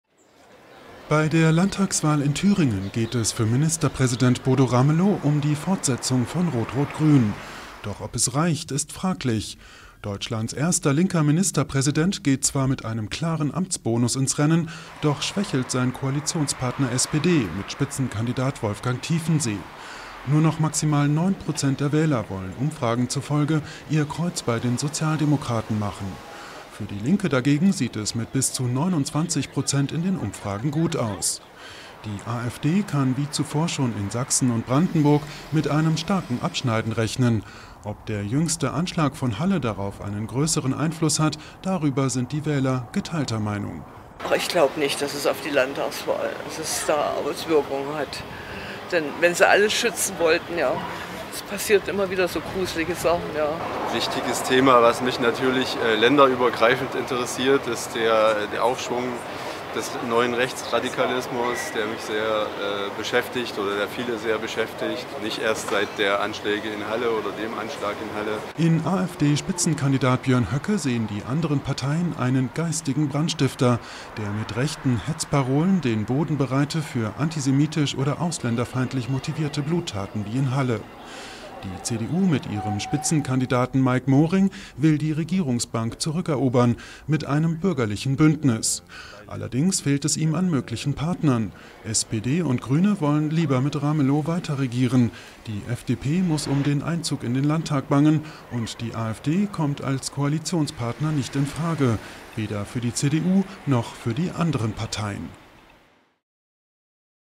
Wahl in Thüringen: Ramelow kämpft um Rot-Rot-Grün | AFP
In Thüringen kämpft Deutschlands erster linker Ministerpräsident Bodo Ramelow um die Fortsetzung der rot-rot-grünen Koalition. Der 63-Jährige dürfte bei der Landtagswahl von seinem Amtsbonus profitieren, allerdings schwächelt der Bündnispartner SPD. Die AfD kann wie zuvor schon in Sachsen und Brandenburg mit einem starken Abschneiden rechnen. IMAGES AND SOUNDBITES